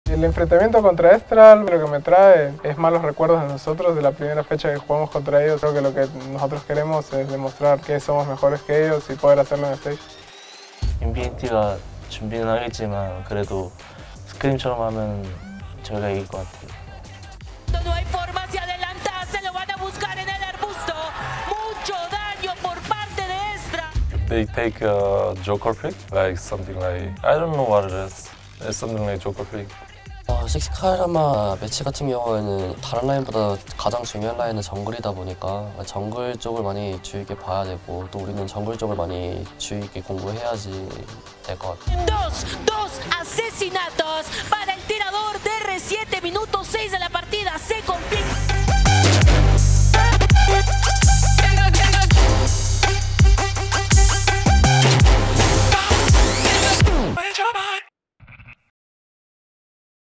¿Quién va a la Final y quién dice adiós? | Adelanto Playoffs LLA Semana 3 | League of Legends
Mientras que el primer finalista del Apertura 2023 se definirá entre Six Karma y Movistar R7, Estral e INFINITY chocarán para ver quién se queda fuera del camino y eliminado de los Playoffs LLA.

No te pierdas las emociones de la penúltima semana de playoffs este martes y miércoles